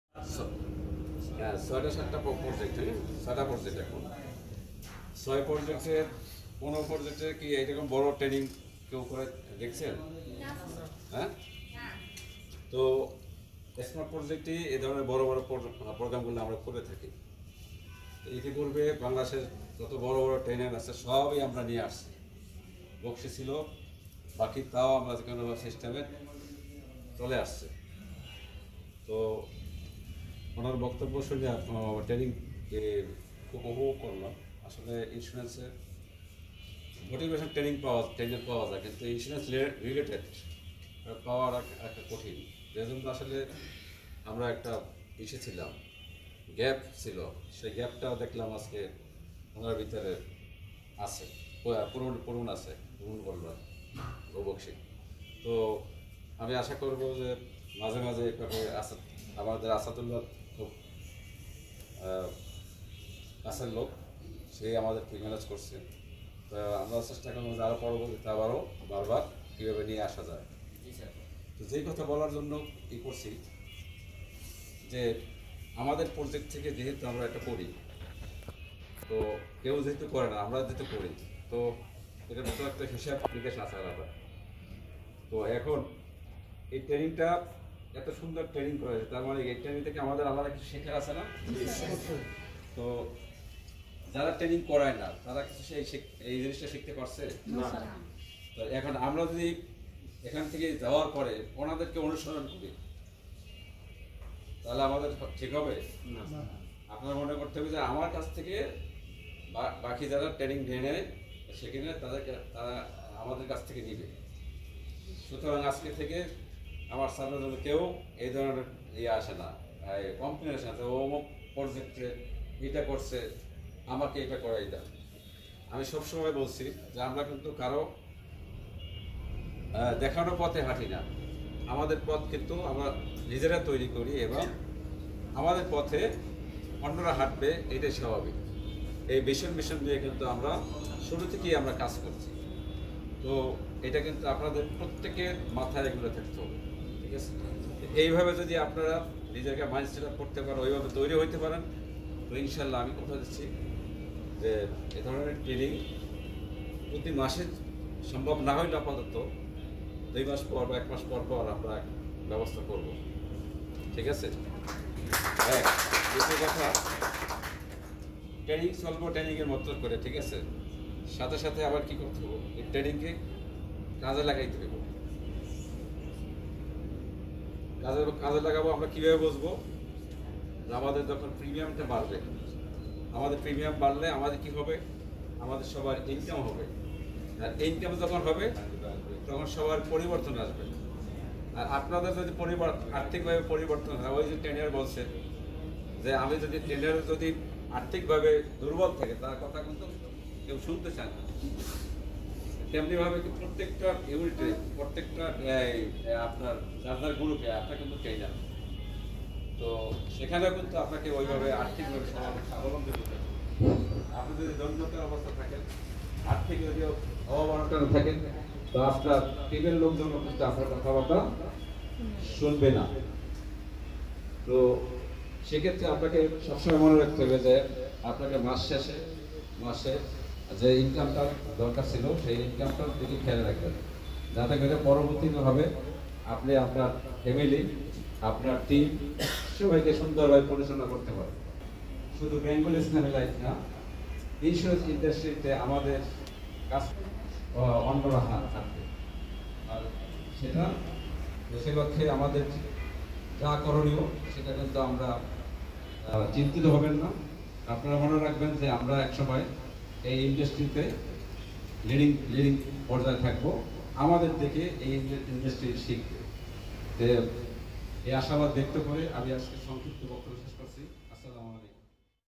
বেঙ্গল ইসলামি লাইফ ইন্সুইরেন্স কোম্পানী রিডার্স কনফারেন্সে ডিরেক্টর ব্রিফিং দিচ্ছেন, mcbtv
বেঙ্গল ইসলামি লাইফ ইন্সুইরেন্স কোম্পানী রিডার্স কনফারেন্স